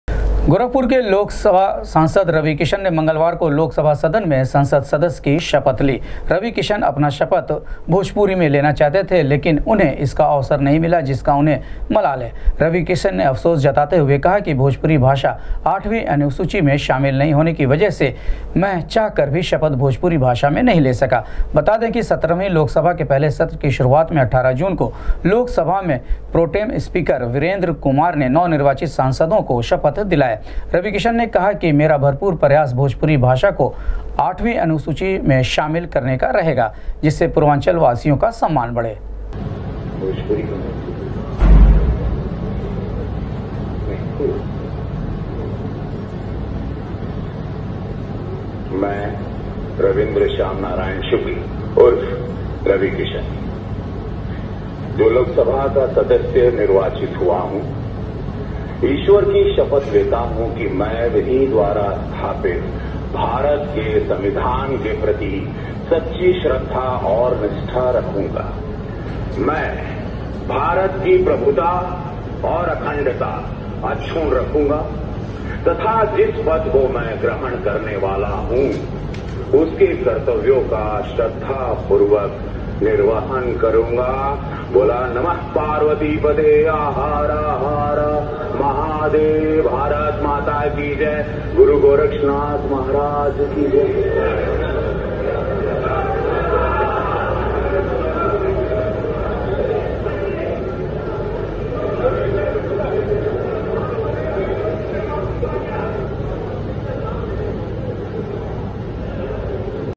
गोरखपुर सदर लोकसभा के नवनिर्वाचित सांसद रविकिशन ने लिया संसद सदस्य की शपथ
गोरखपुर सदर लोकसभा के नवनिर्वाचित सांसद रविकिशन ने लिया संसद सदस्य की शपथ, कहा भोजपुरी भाषा को  8 वीं अनुसूची में शामिल करने का रहेगा मेरा भरपूर प्रयास